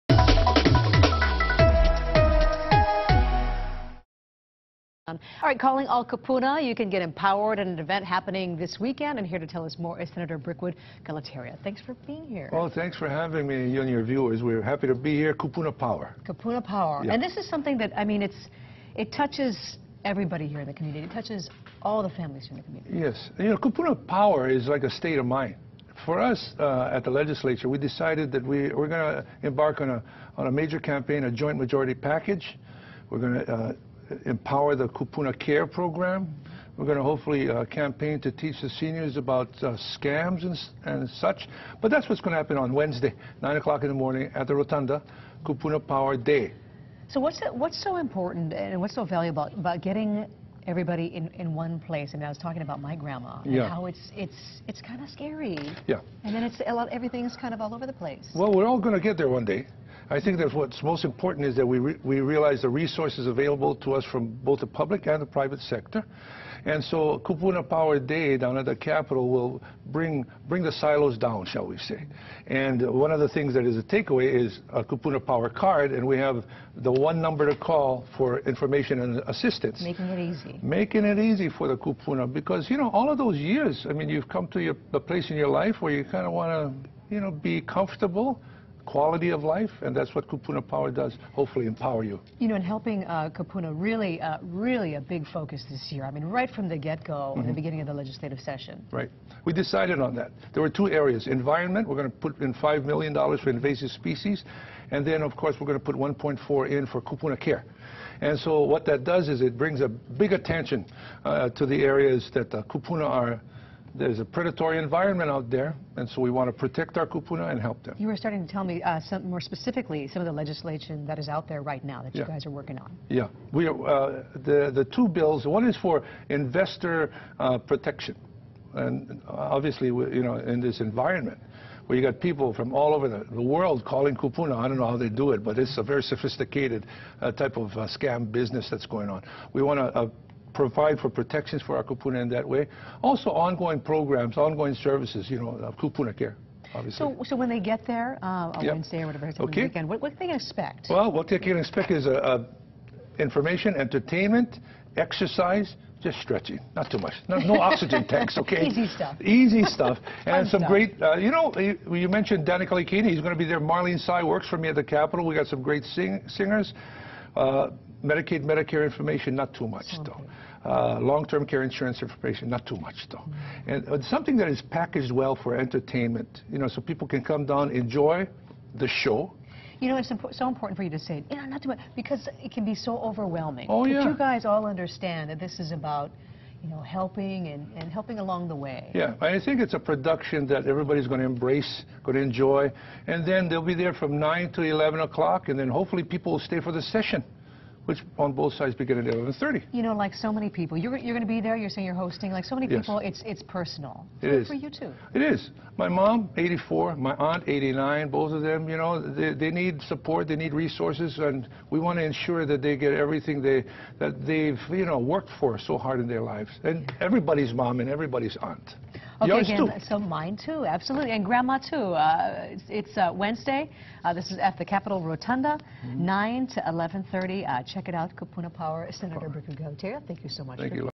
Rally for Kupuna power at the Capitol!
Sen. Brickwood Galuteria talks about Kupuna Power Day at the State Capitol on Wednesday morning. For news you need to know,